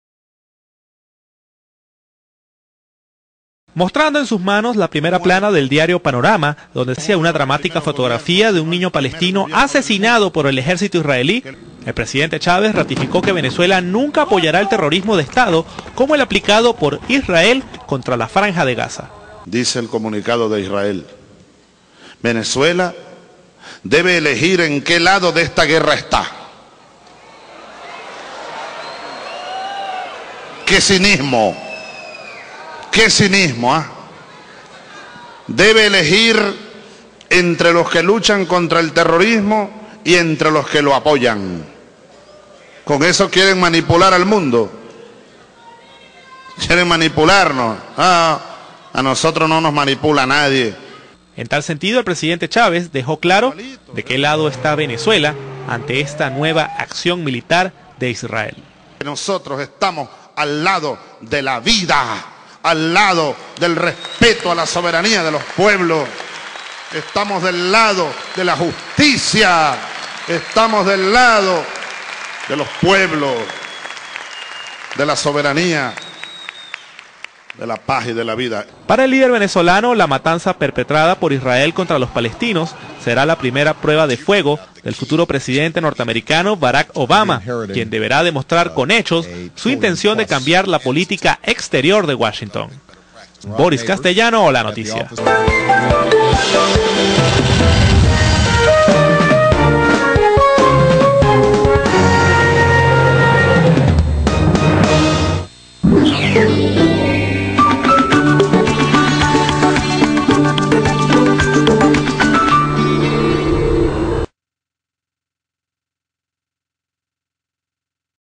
7 Ene 2009 Venezuela está del lado de la vida, la justicia y soberanía de los pueblos
Venezuela está del lado de la vida, la justicia y soberanía de los pueblos.